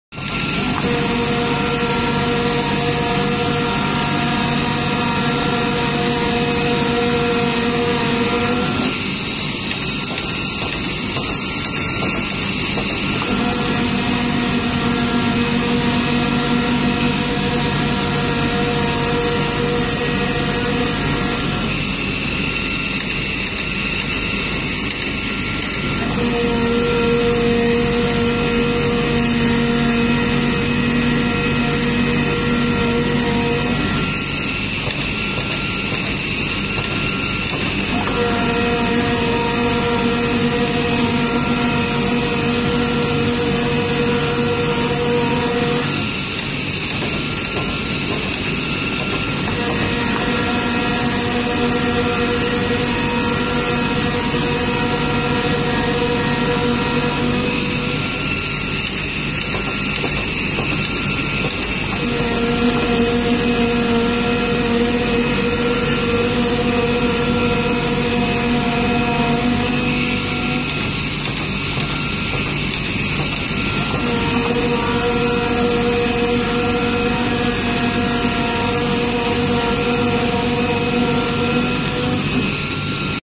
ITTA IC650DH demo cutting shoes design (Fast cutting speed 37pcs 5 mins)
ITTA IC650DH demo cutting leather shoes design it takes 16 seconds to cut 1 piece (Fast cutting speed 37pcs 5 mins), the notch show not sharp as photo.

PT Barunesia Jayadi
Contact Person : Mrs. Linda, Ms. Betha